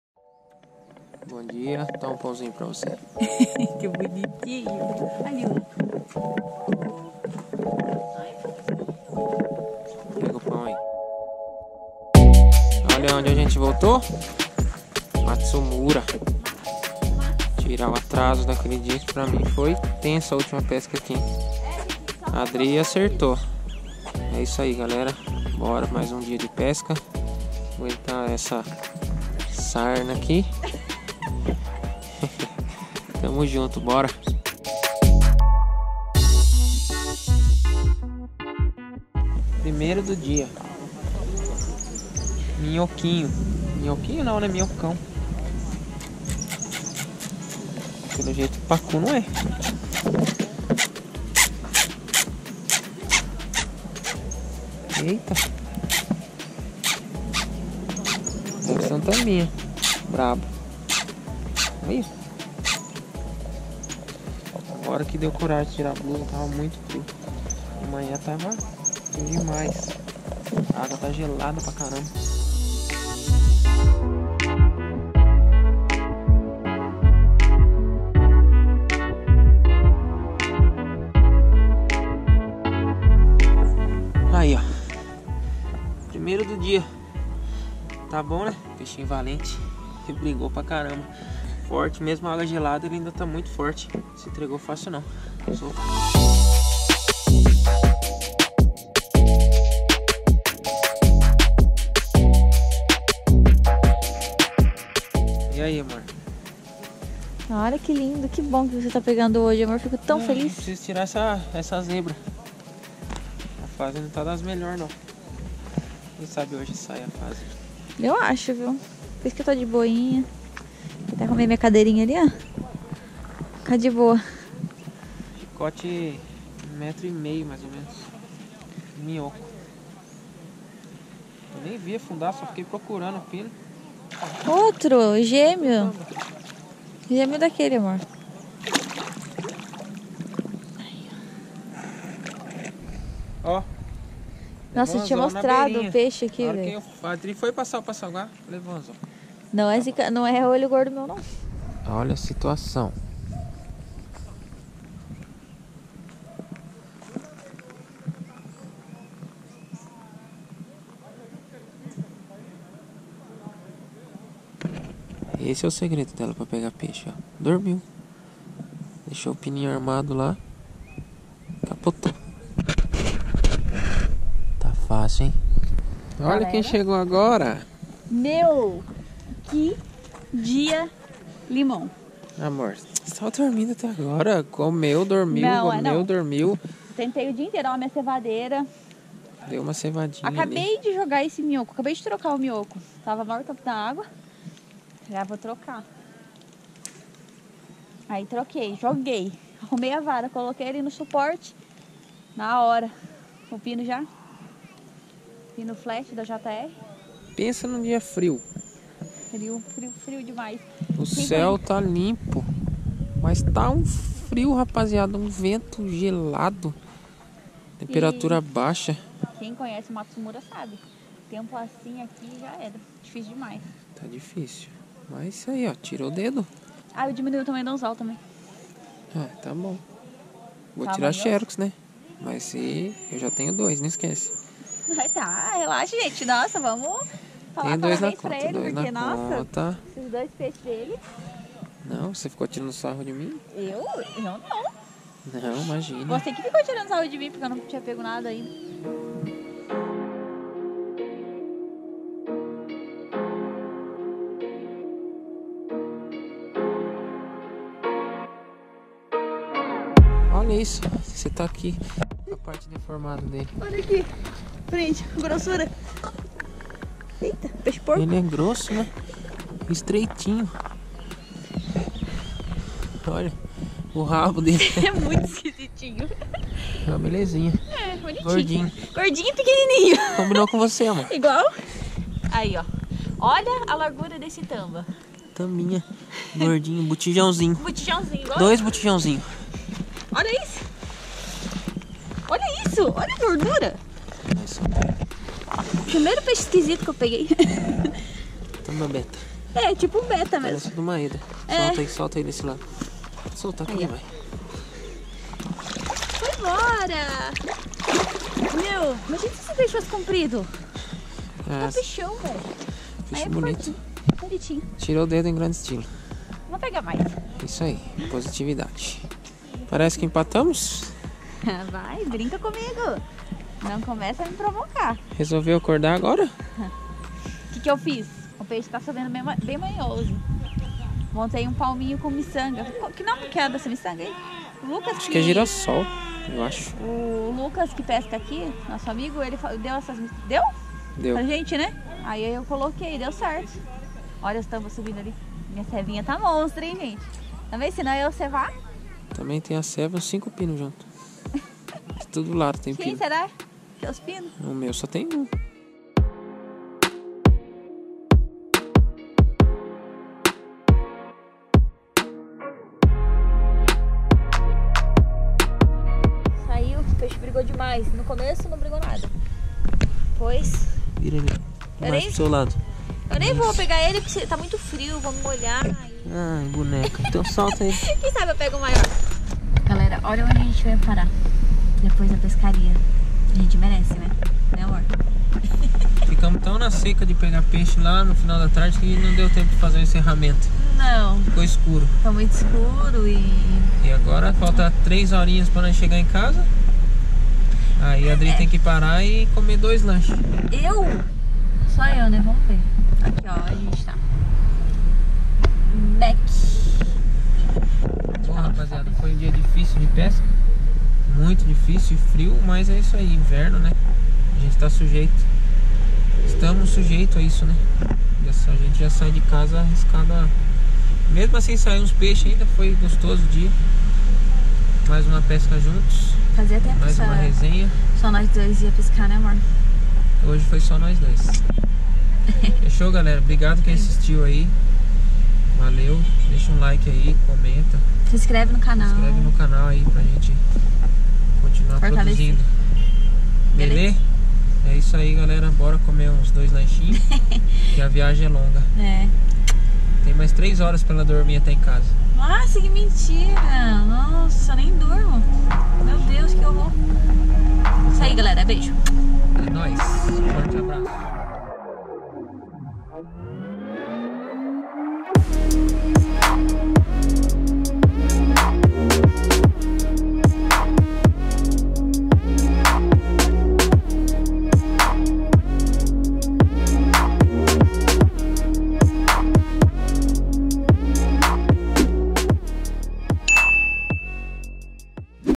PESCARIA HARD NO FRIO E LIMONADA GARANTIDA !
O inverno chegou mas não deixamos de pescar, mesmo sabendo que a dificuldade tende a aumentar, fomos atrás dos grandes peixes do Pesqueiro Matsumura novamente. O resultado vocês acompanham no decorrer do vídeo.

Equipamentos utilizados:
Vara Shore Jig Slugger 2,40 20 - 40 LBS, Vara Pitton 2.70, Tanka By Nakamura 8'0'' (2,43m) 40-80lbs e 9'0'' (2,73m) 50-100lbs, Sucuri 2,70. Carretilhas MARURI Django, TNT 7 e Brahva XL.